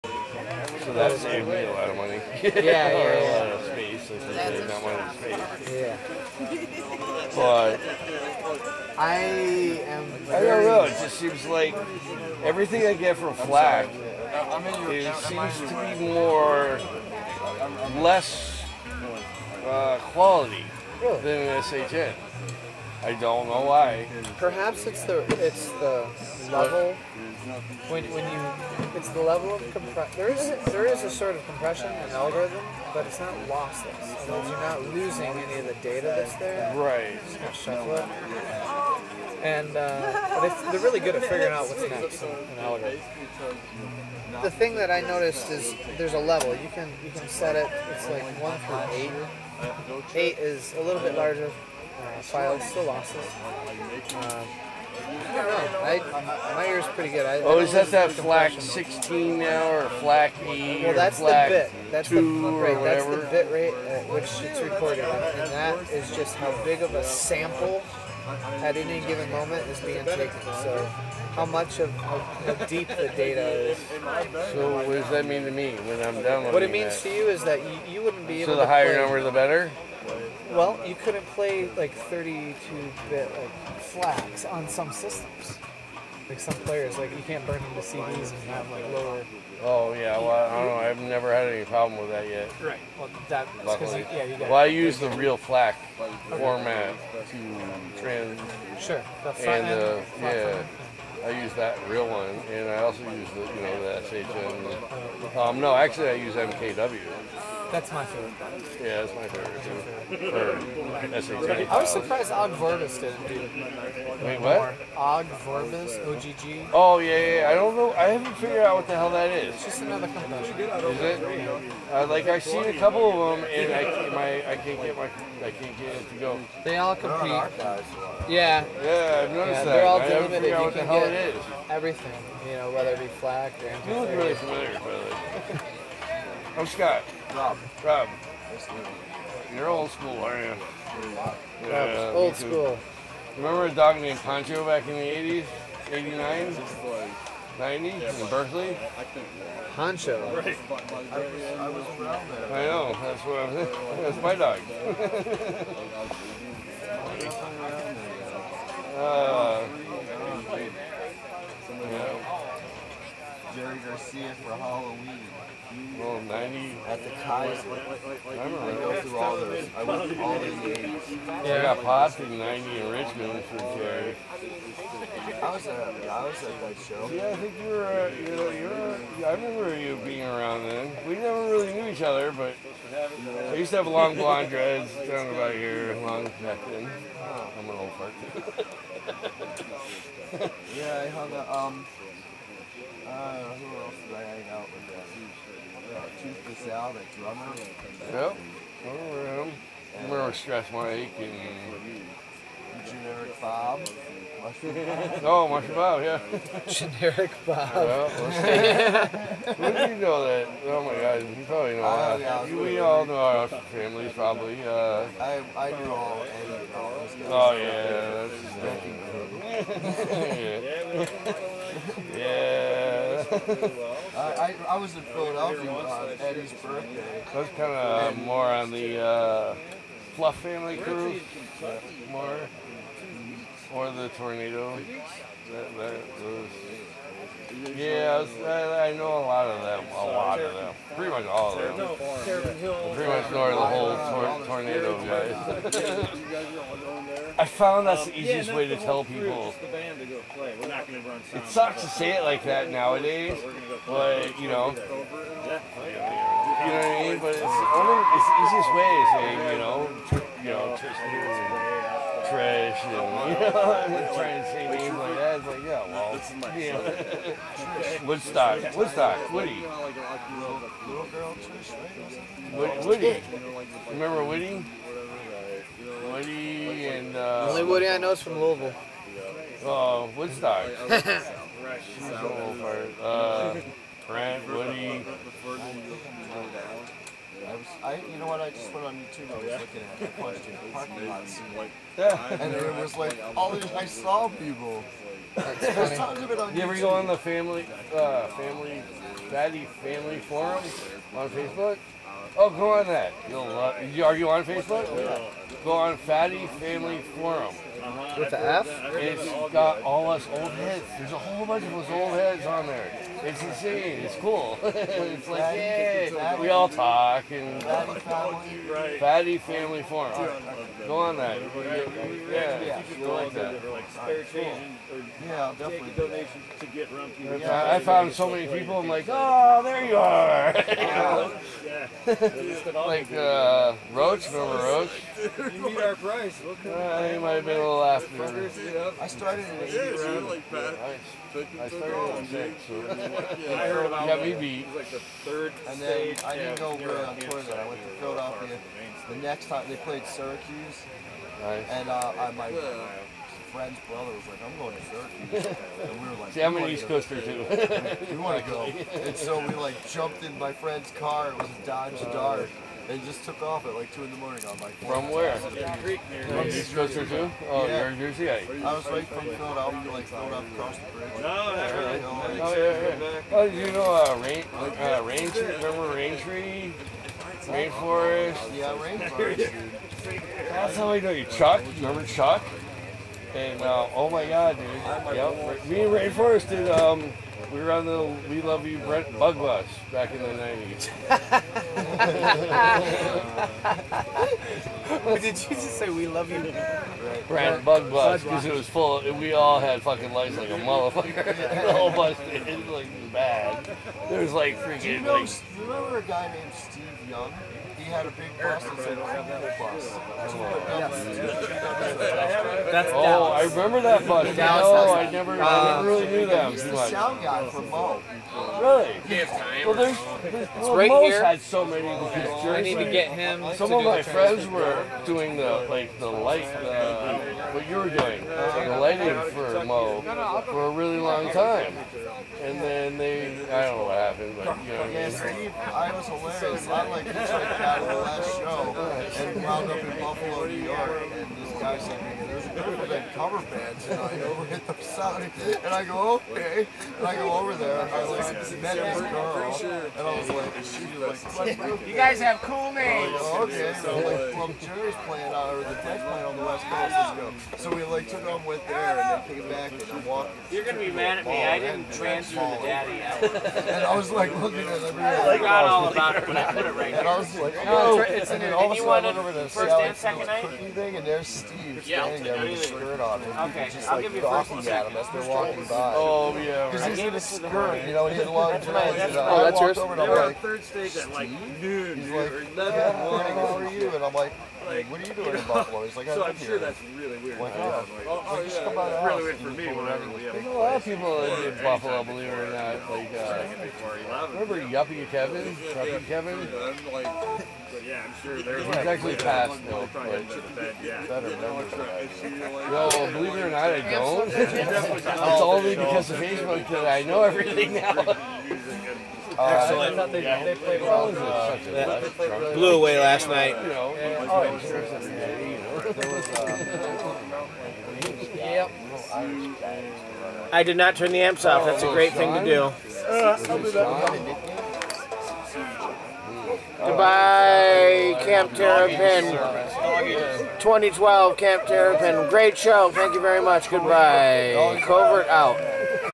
00470 Terrapin Flyer at Camp Terrapin 2012 10 of 10
Amazing vibes at the fest~
August 25, 2012
Stonehouse Park
Earlville, IL
Terrapin Flyer
wsg: Mark Karan, Tom Constanten and some great sit ins!
Covert Productions